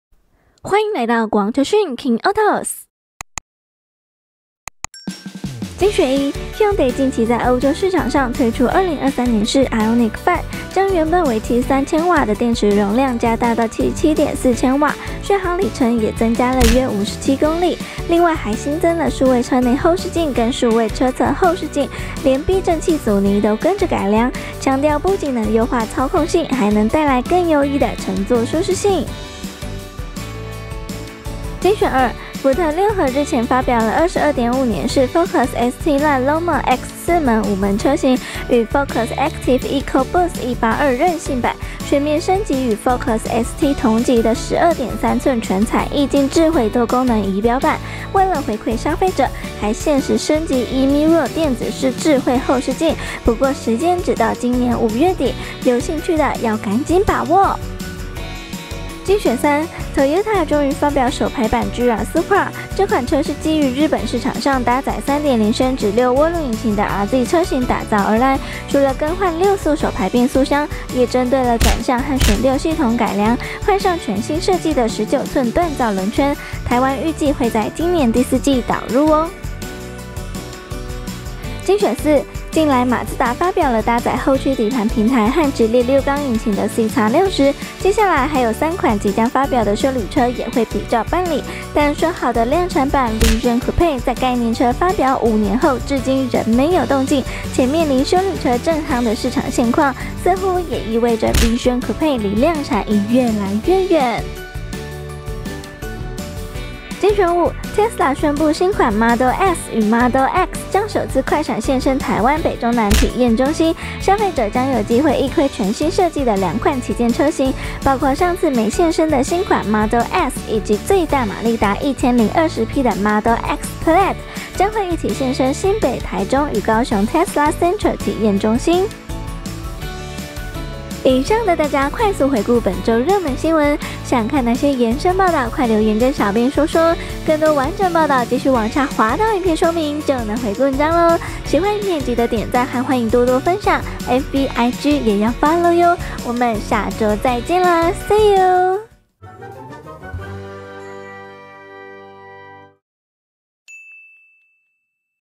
《國王車訊-熱門週報》暢銷人氣王22.5年式《Ford Focus》正式發表!!!
本週熱門新聞５則Get！跟我們看下去吧👇👇👇

歐規2023年式《Hyundai Ioniq 5》換上77.4kWh電池 車內外後視鏡變成數位的

售價漲1.9萬元！22.5年式《Ford Focus》升級數位儀表板 5月底領牌贈電子智慧後視鏡

六速手排《Toyota GR Supra》第四季抵台 全車系換上全新可變阻尼避震系統

市場休旅車正熱 《Mazda  》量產計畫恐成泡影

上回沒看到的《Model S》終於來了 《Tesla》宣布明日起偕同《Model X Plaid》將舉辦全台展示活動